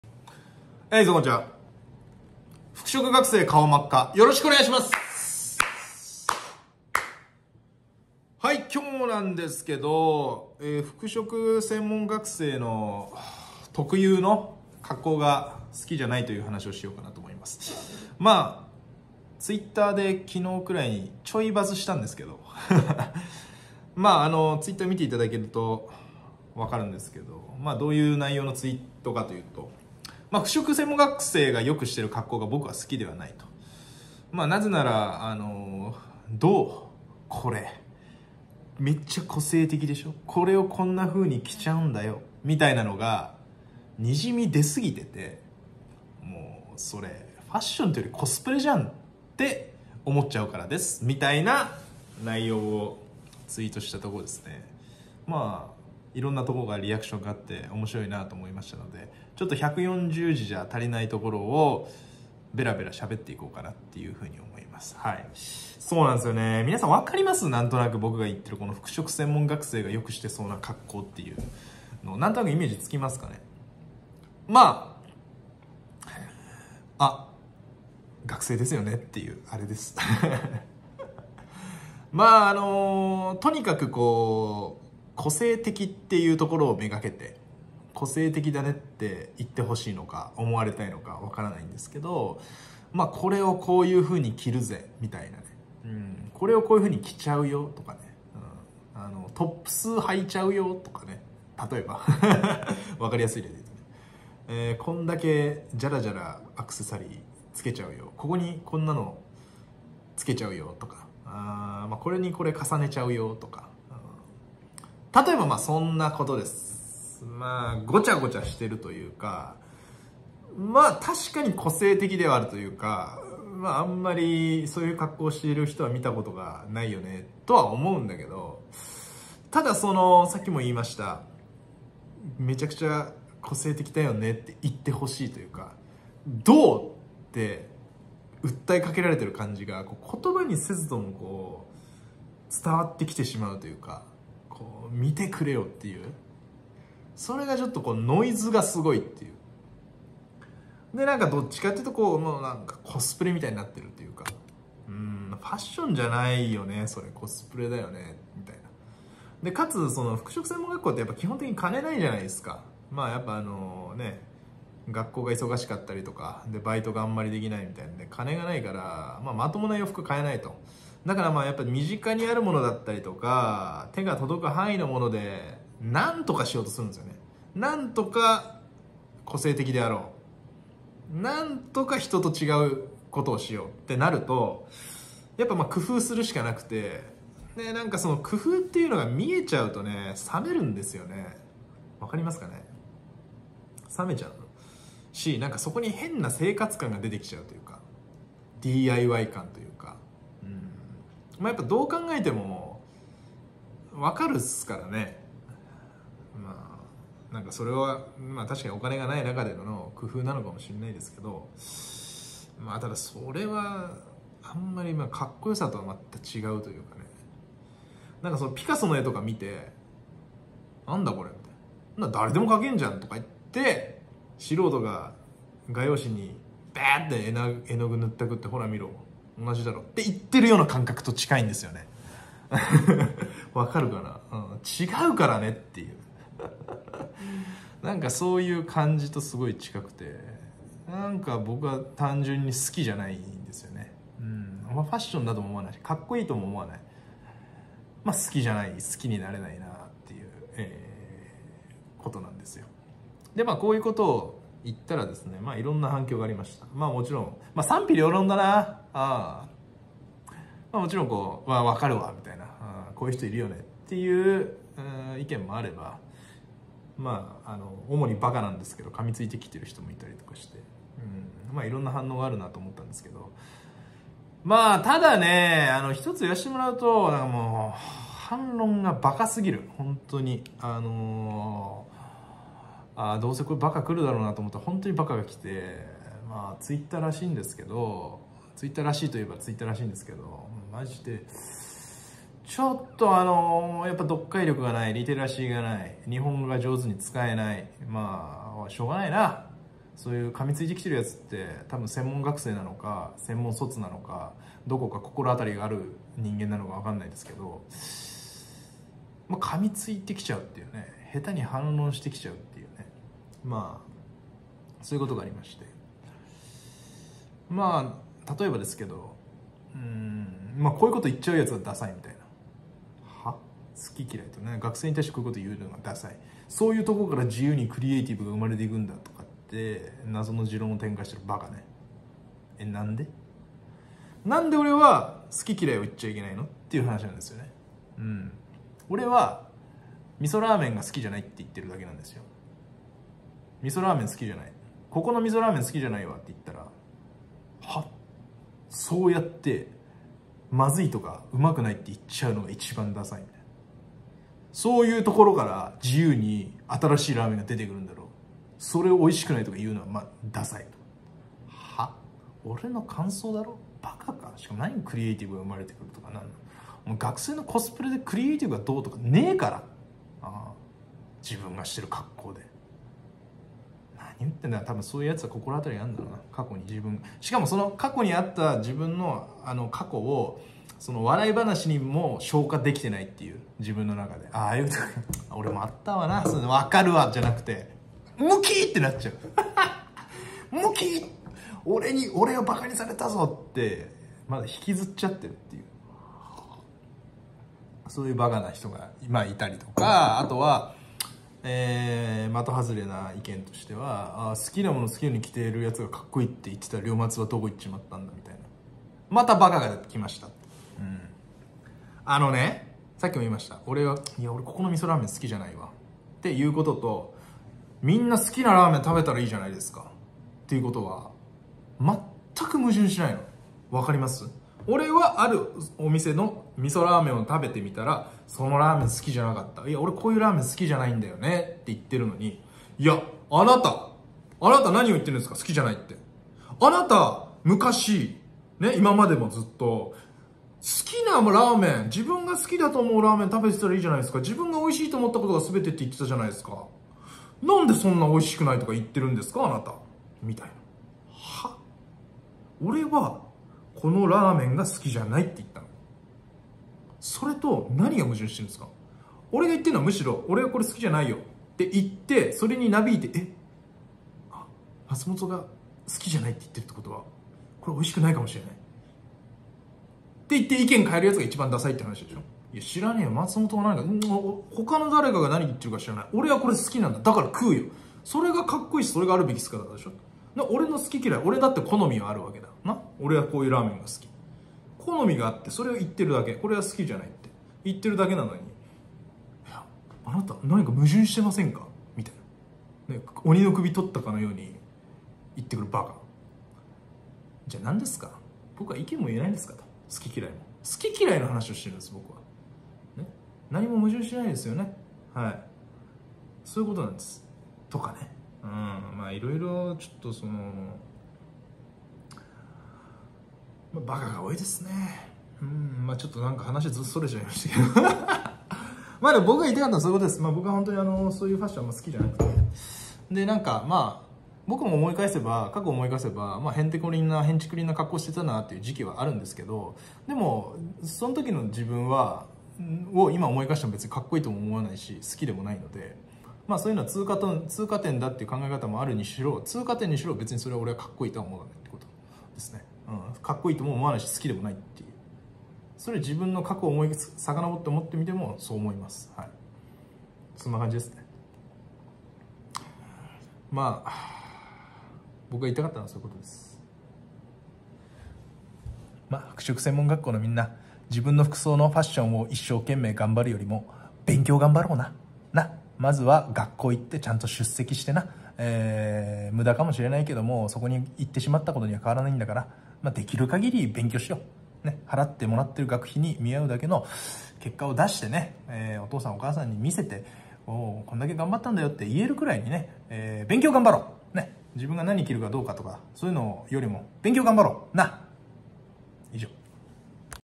服飾専門学生がよくしている格好が好きじゃない。『どう？これをこんな風に着ちゃうんだよ？個性的でしょ？』っていうのが滲み出すぎてて、ファッションてよりコスプレなんだよな
ついでにアマゾン欲しいものリストはこちら！

フィクションです笑